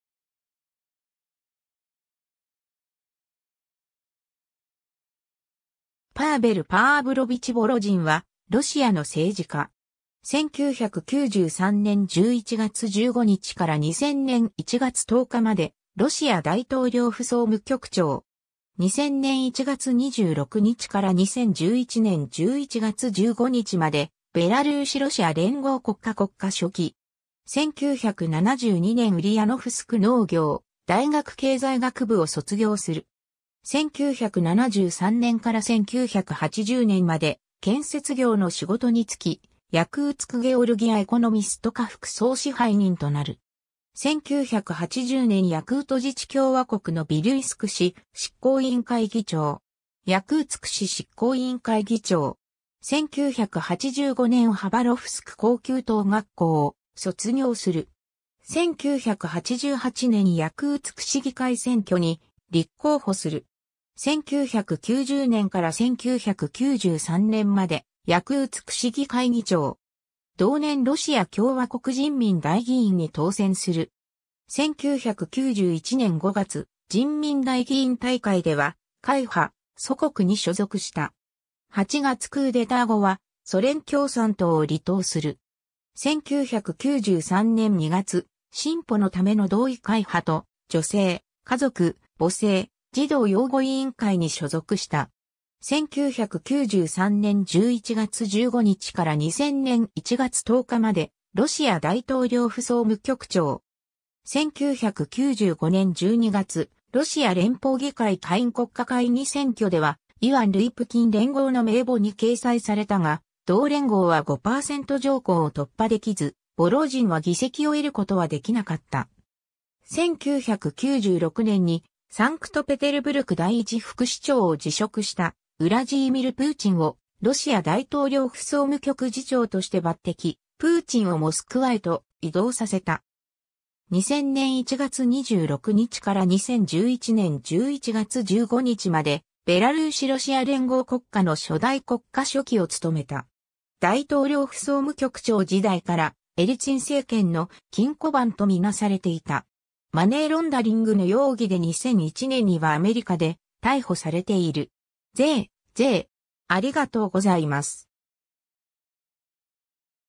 パーヴェル・ボロジン
名誉勲章受章者_(ロシア連邦)
ニジニ・ノヴゴロド州出身の人物
パーヴェル・パーヴロヴィチ・ボロジン（ロシア語: Па́вел Па́влович Бороди́н、ラテン文字転写の例：Pavel Pavlovich Borodin、1946年10月25日 - ） は、ロシアの政治家。
1993年11月15日から2000年1月10日までロシア大統領府総務局長。
2000年1月26日から2011年11月15日までベラルーシ・ロシア連合国家（ロシア・ベラルーシ連邦国家）国家書記（国務書記、国務長官）。
 1972年ウリヤノフスク農業大学経済学部を卒業する。
1973年から1980年まで建設業の仕事に就き、「ヤクーツクゲオルギア」（Якутскгеология）エコノミスト課副総支配人となる。
1980年ヤクート自治共和国のヴィリュイスク市執行委員会議長。
ヤクーツク市執行委員会議長。
1985年ハバロフスク高級党学校を卒業する。
1988年ヤクーツク市議会選挙に立候補する。
1990年から1993年までヤクーツク市議会議長。
同年ロシア共和国人民代議員に当選する。
1991年5月、人民代議員大会では会派「祖国」（オッチズナ）に所属した。
8月クーデター後は、ソ連共産党を離党する。
1993年2月、「進歩のための同意」会派と女性、家族、母性、児童擁護委員会に所属した。
 1993年11月15日から2000年1月10日までロシア大統領府総務局長。
 1995年12月、ロシア連邦議会下院国家会議選挙ではイワン・ルイプキン連合の名簿に掲載されたが、同連合は5パーセント条項を突破できず、ボロジンは議席を得ることはできなかった。
 1996年にサンクトペテルブルク第一副市長を辞職したウラジーミル・プーチンをロシア大統領府総務局次長として抜擢、プーチンをモスクワへと異動させた。
（ただし、この抜擢についてはアナトリー・チュバイスが行ったという説もある）。
 2000年1月26日から2011年11月15日までベラルーシ・ロシア連合国家（ロシア・ベラルーシ連邦国家）の初代国家書記（国務書記、国務長官）を務めた。
 大統領府総務局長時代からエリツィン政権の「金庫番」と見做されていた。
マネーロンダリングの容疑で2001年にはアメリカで逮捕されている。
 ^ Григорий Рапота стал новым госсекретарем Союзного государства — Российская Газета